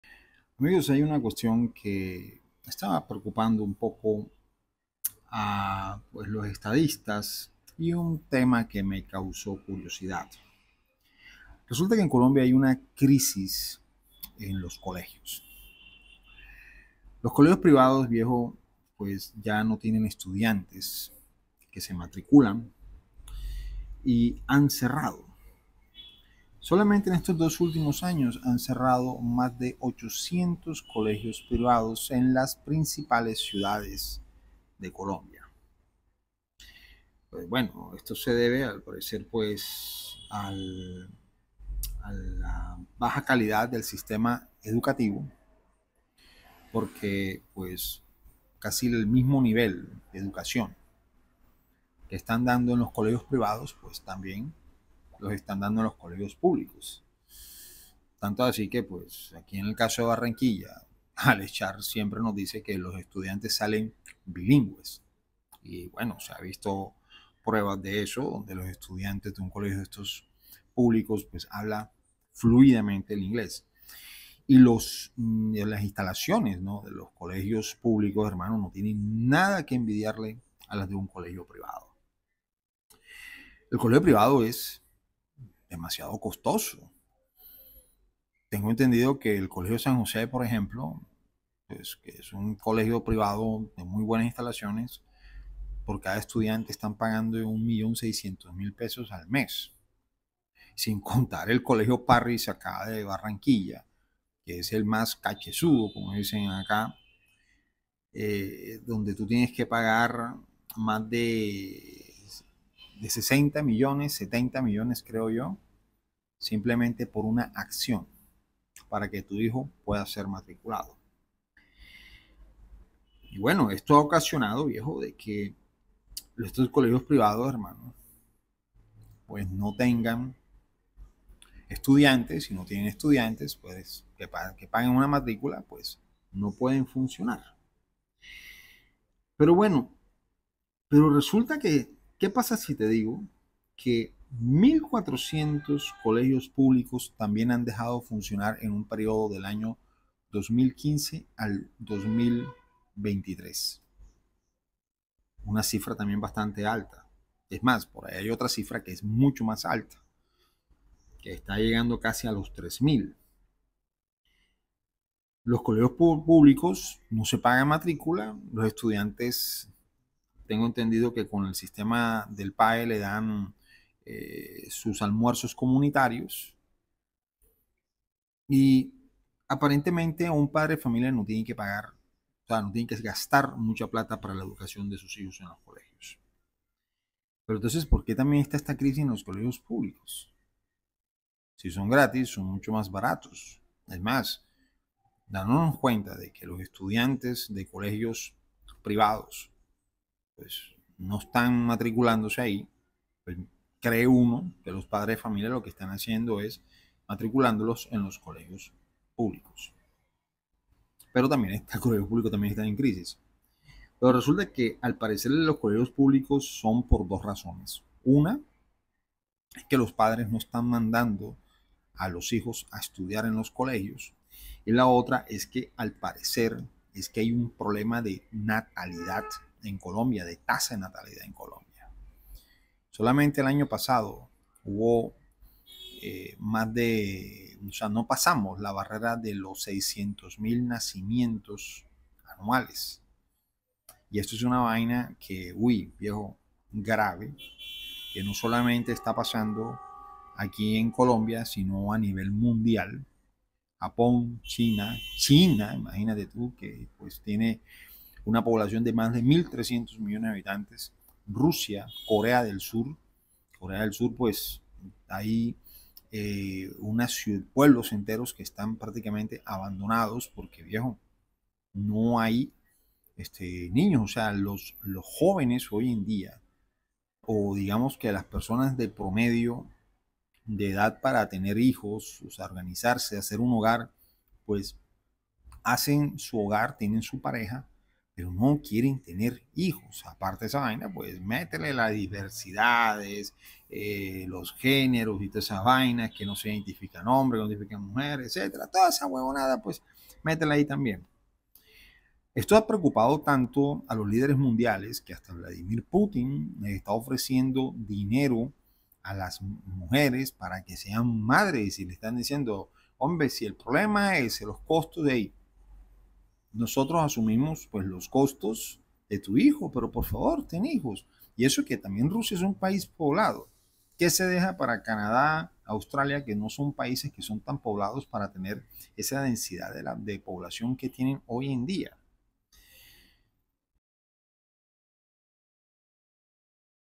Crisis financiera en la educación en Colombia
Muchos colegios privados han cerrado, ya que los estudiantes no se matriculan. Pero lo que llama la atención es que eso mismo pasa en los colegios públicos, donde la matrícula es cero. ¿Qué está pasando entonces?